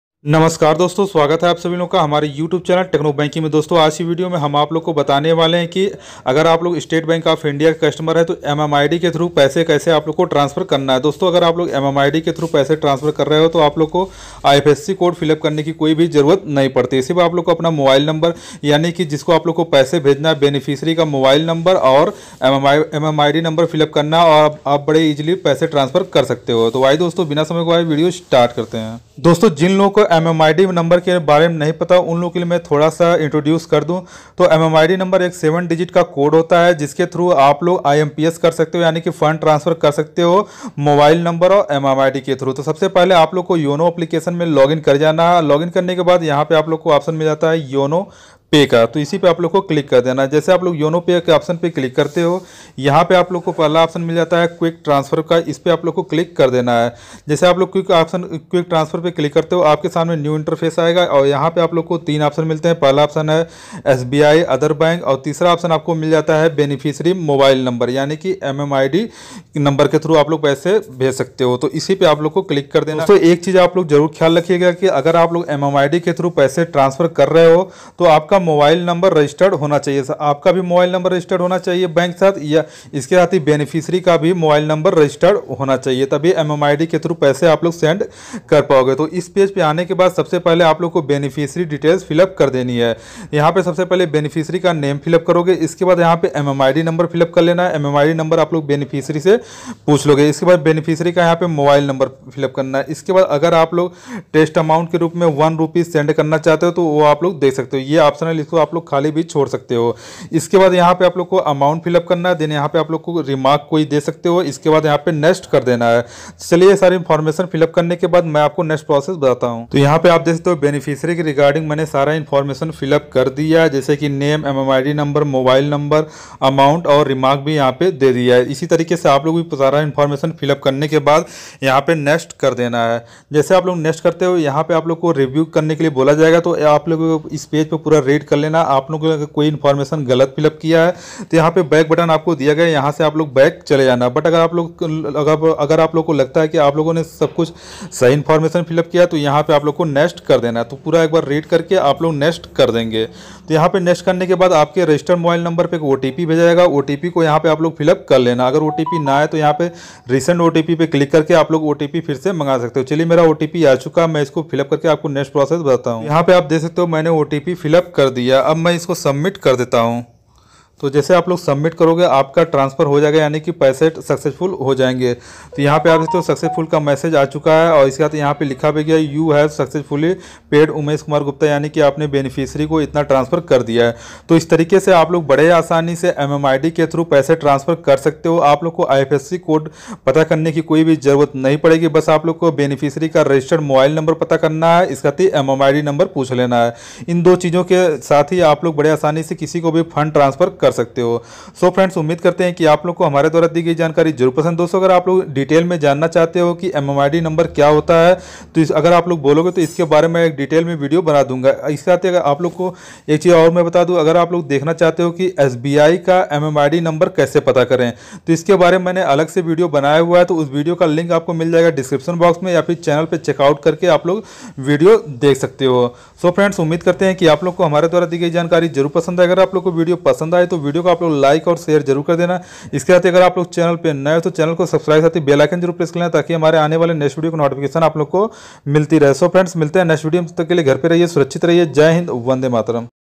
SBI YONO - MMID No Aur Mobile Number Se Paise Kaise Transfer Kare| Transfer Money Through MMID No
aaj ke video me maine bataya hai ki sbi yono se mmid number aur mobile number se paise kaise transfer kare.

Topic Covered.

how to transfer money through mmid number
how to transfer money through mobile number

username- mauryapk

username- @technobankingbypraveen

username- @pkmaurya123

by

techno banking
technobanking